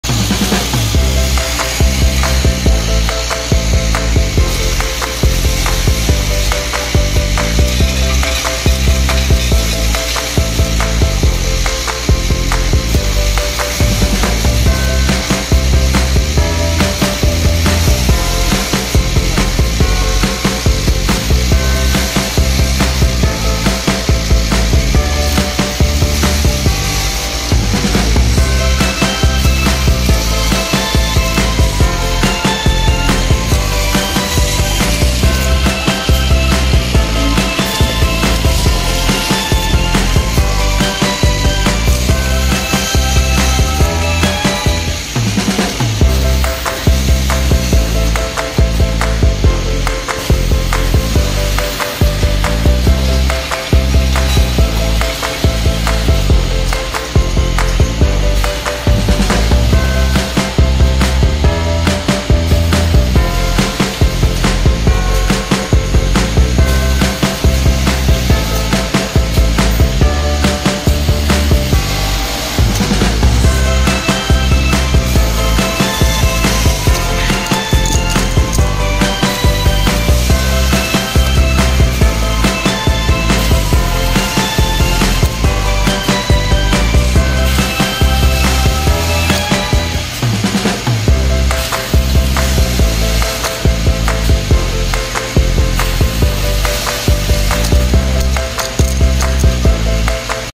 Poker Chips Pad Printing Machine Automatic
It use for print poker chips, can print both sides at once, with uv curing system. The process is fully automatic.

More detail will provide if you needed, here is contact way:
Cindy